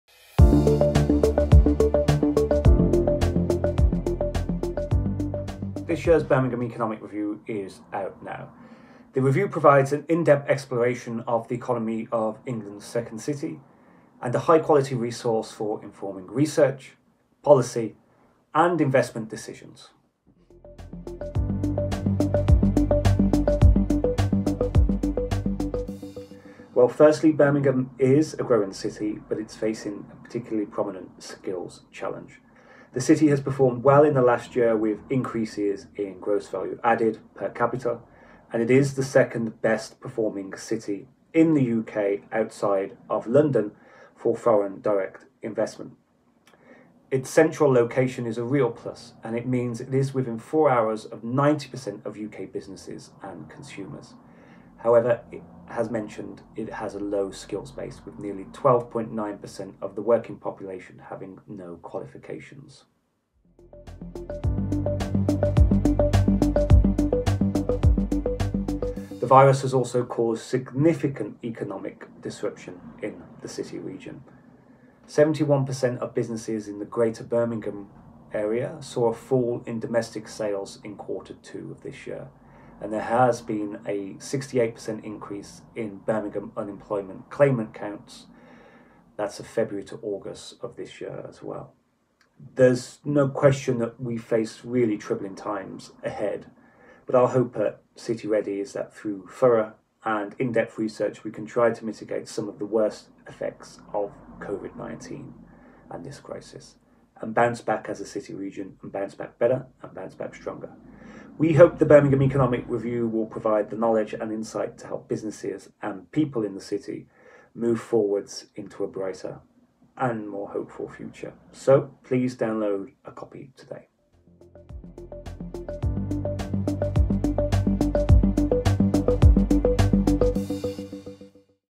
Birmingham Economic Review 2020
The annual Birmingham Economic Review is produced by the University of Birmingham’s City-REDI and the Greater Birmingham Chambers of Commerce, with contributions from the West Midlands Growth Company. It is an in-depth exploration of the economy of England’s second city and a high-quality resource for informing research, policy and investment decisions.